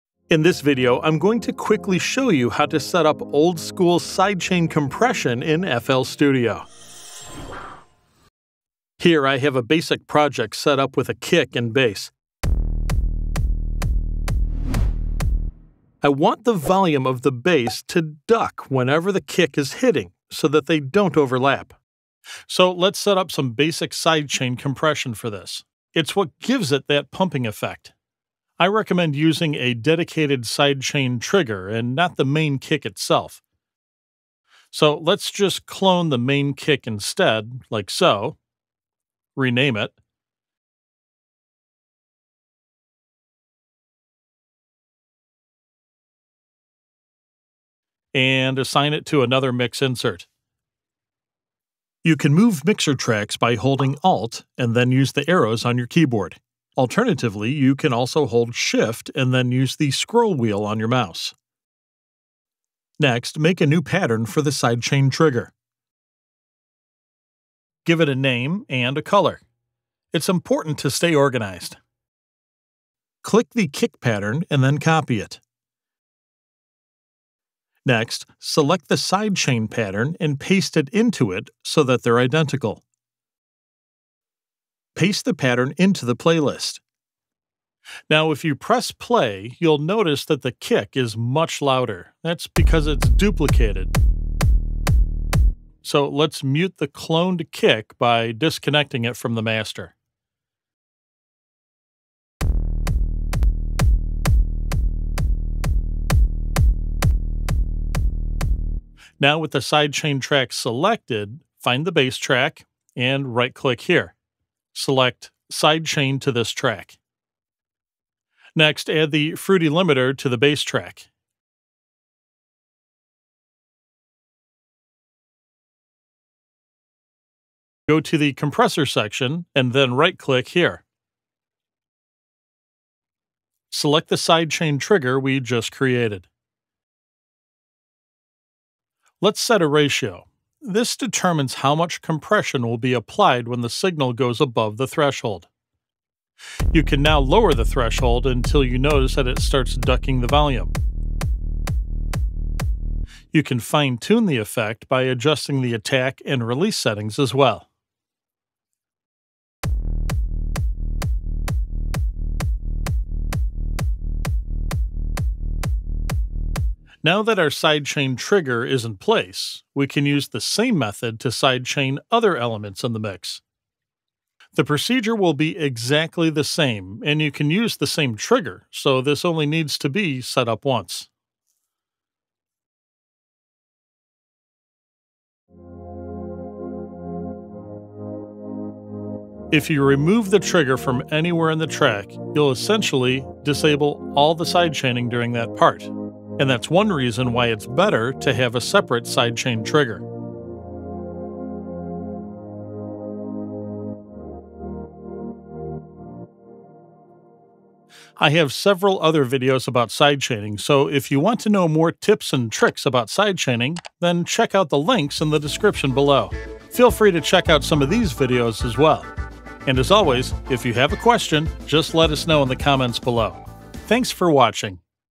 How To Sidechain In FL Studio (Beginner Tutorial)
In this FL Studio beginner tutorial, you'll learn how to set up proper sidechain compression in FL Studio 20. Sidechaining is an important mixing tool that allows you to control the overlap in the low end. By sidechaining your bass, for example, you can ensure that it doesn't overlap with your kick. That way you'll get a much cleaner mix. There are many ways to sidechain in FL Studio, but this is the old-school way with a trigger and a compressor. In this FL Studio beginner tutorial I will show you how to set up a sidechain trigger, and how to use the fruity limiter to duck the volume of the bass, using the kick pattern. Sidechaining can be used for a lot of things, but it's most commonly used for so called volume ducking. In modern EDM almost everything will tend to be sidechained. This allows the main kick to really punch through the mix, and prevents the low-end from sounding muddy or muffled.
You can also sidechain other elements in your mix, like pads, leads, strings and so on. A good rule of thumb is to apply very aggressive sidechaining (volume ducking) on low-end elements like your bass, and then use less sidechaining as the frequency of the sound goes up. For example, you don't need to sidechain hats and other high-end elements. Vocals should typically also not be sidechained. Midrange elements on the other hand, like chords and such will typically be somewhat sidechained.

00:00 Introduction
00:09 How to sidechain in FL Studio 20
00:28 Use a dedicated Sidechain trigger
00:51 How to move mixer tracks in FL Studio
01:02 make a dedicated sidechain pattern
02:05 How to sidechain with fruity limiter

🎹 Do you need professional feedback on your music? Or perhaps mastering?